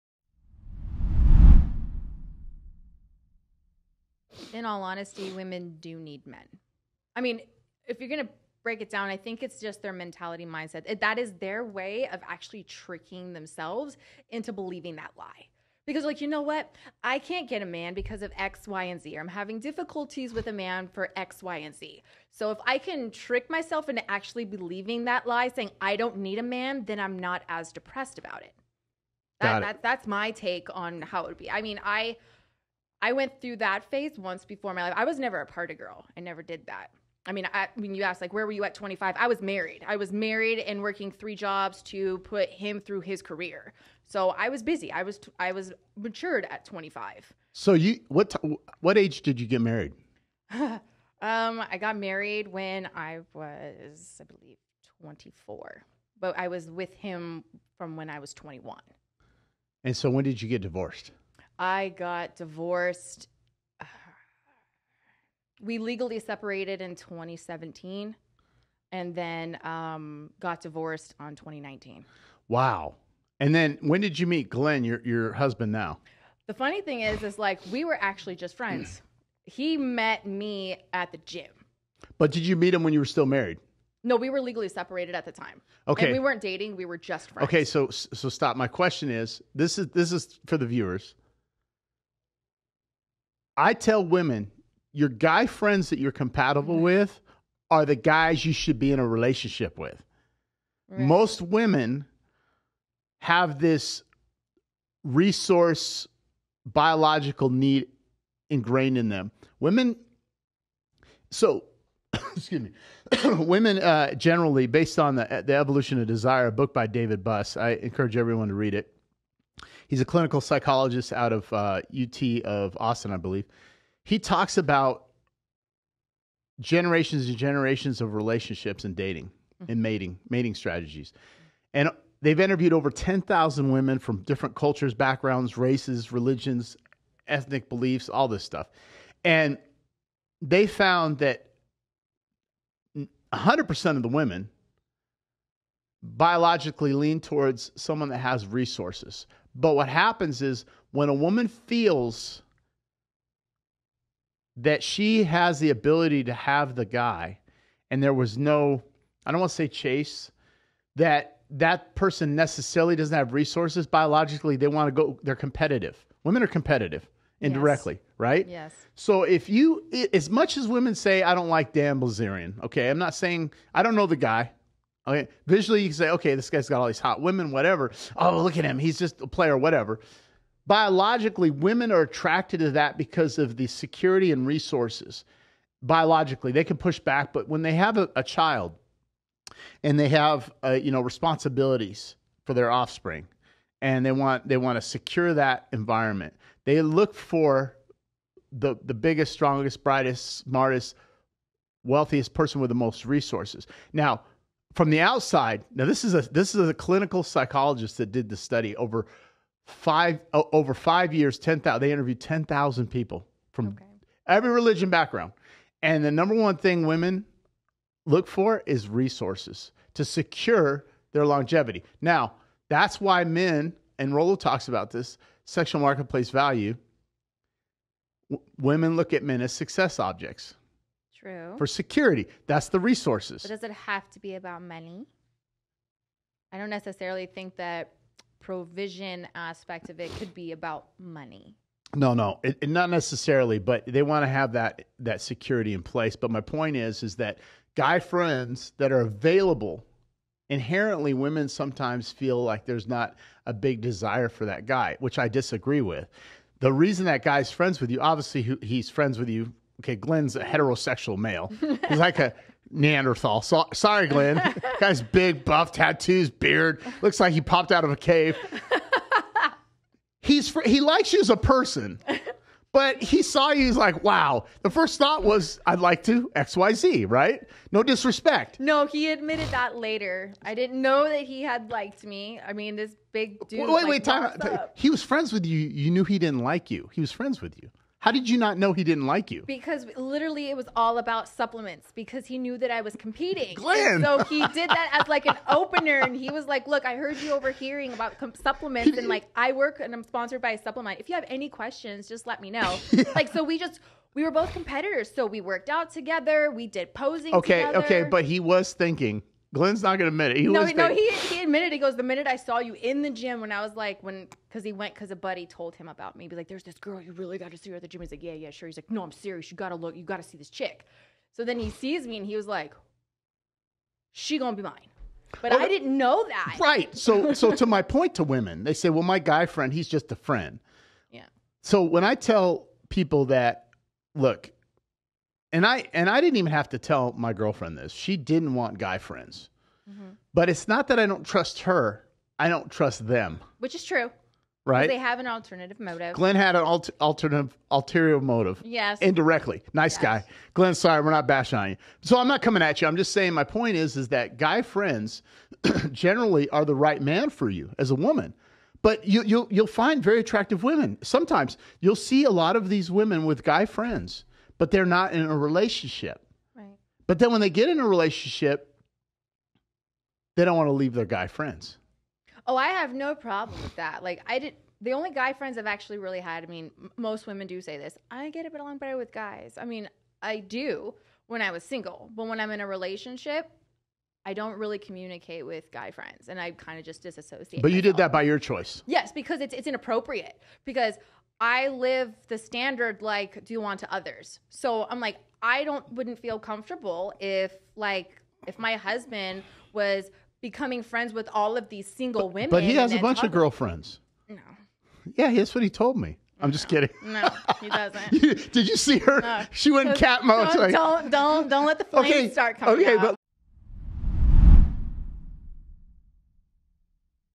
Do Women Really Need a Man | Hypergamy
Coco is married to the Podcaster Glenn Lawrence and has launched her own Podcast, her thoughts on relationships, dating, and marriage. From Episode #60